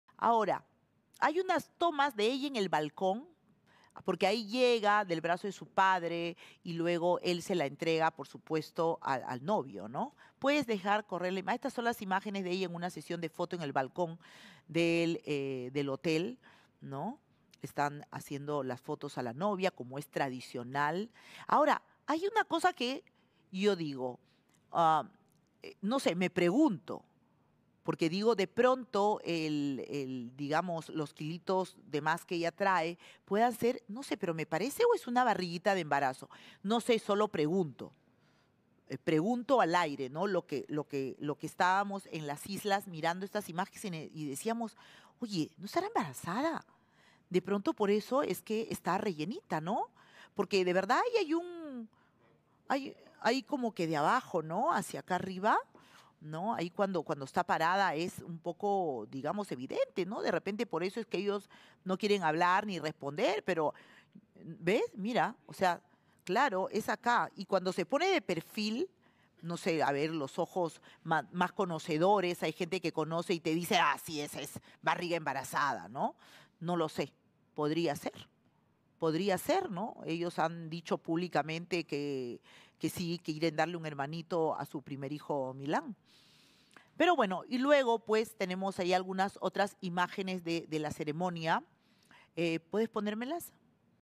Cassandra Sánchez: ¿Lució una pancita de embarazada el día de su boda?
La sesión de fotos que tuvo la novia dejó múltiples dudas.